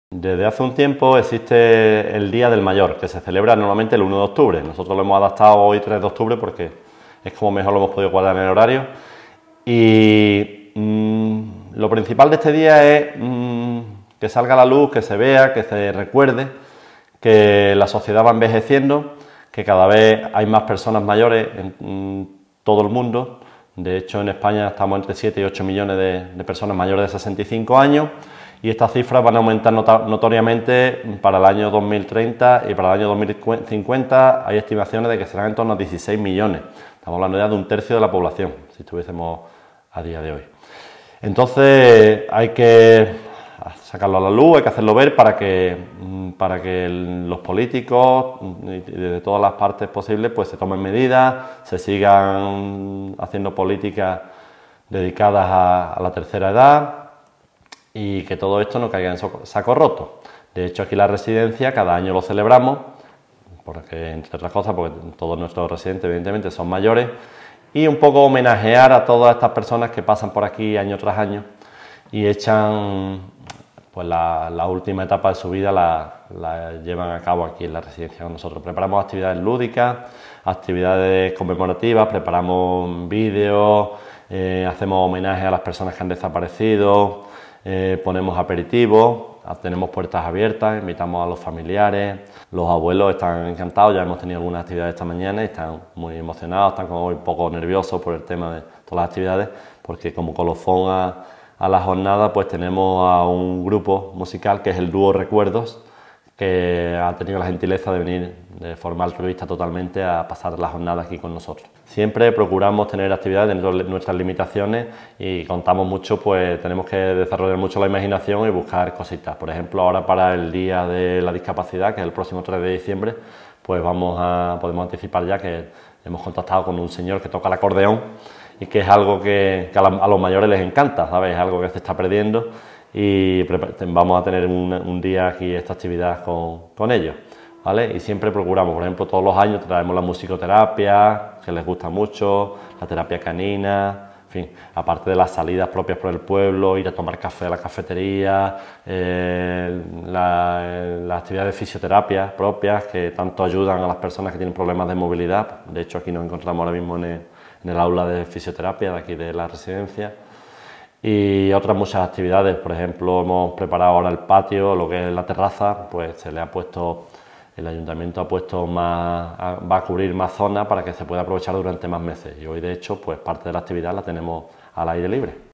LA RESIDENCIA DE ANCIANOS DE FARAJÁN CONMEMORA EL DÍA DEL MAYOR
Con motivo del 1 de octubre, Día Internacional del Mayor, la Residencia de Ancianos “Valle del Genal” de Faraján ha querido rendir hoy su particular homenaje a este sector de la población y, en concreto, a todos sus usuarios.
     El Día Internacional del Mayor fue proclamado como tal por las Naciones Unidas en el año 1990 con la intención de concienciar sobre la discriminación social que amenazaba a las personas de mayor edad. Arturo Flores, trabajador social del centro, ha recordado que la sociedad va envejeciendo progresivamente. Muestra de ello es que en la actualidad España tiene casi 8 millones de habitantes de más de 65 años, cifra que se doblará en las próximas décadas. En este sentido, Flores ha destacado la necesidad de que desde las esferas políticas se continúen tomando medidas dirigidas a mantener y mejorar los derechos y la calidad de vida de los ciudadanos de la tercera edad.
    La conmemoración del Día del Mayor en la Residencia de Ancianos “Valle del Genal” ha consistido en el desarrollo de una serie de actividades de carácter lúdico, aunque también se han llevado a cabo merecidos homenajes, se han proyectado vídeos y, en la magnífica terraza exterior de las instalaciones, todos los usuarios han disfrutado de un aperitivo con música en directo en compañía de familiares y amigos, que se han desplazado al centro para participar en la jornada de puertas abiertas.
     Por último, Arturo Flores ha añadido que además de las jornadas de celebración, la Residencia de Ancianos “Valle del Genal” de Faraján realiza a lo largo del año multitud de talleres que gozan de una gran aceptación entre los residentes, entre ellos destacan algunos realizados últimamente como el taller de cocina, de musicoterapia, de aromaterapia, de recuerdos del pasado, de papiroflexia o la terapia canina.